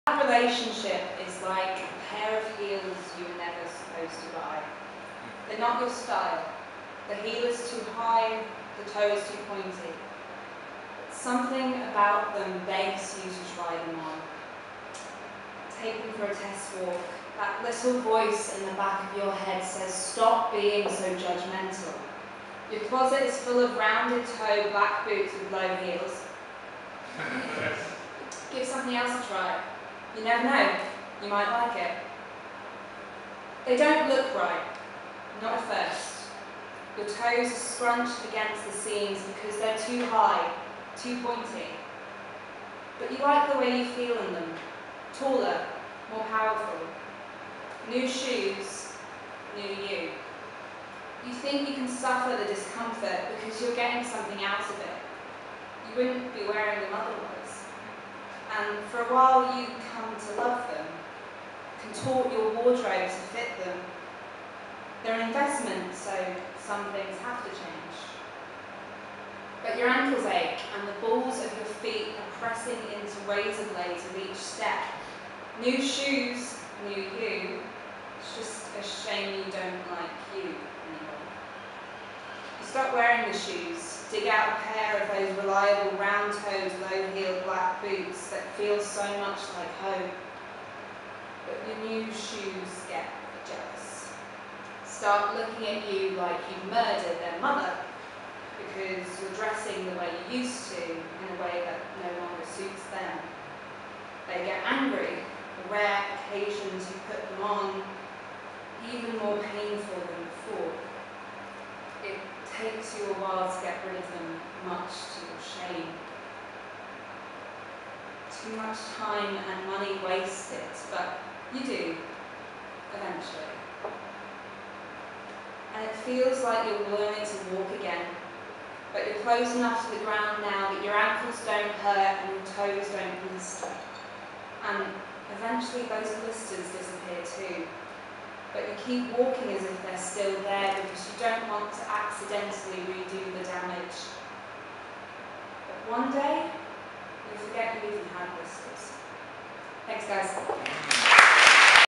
Emma Thompson @ Grizzly Pear
Grizzly Pear is a spoken word night hosted by the University of Birmingham's only creative writing society: Writers' Bloc.

Poetry and prose of all shapes and sizes are performed live in Selly Oak, by both open mic-ers and headliners alike.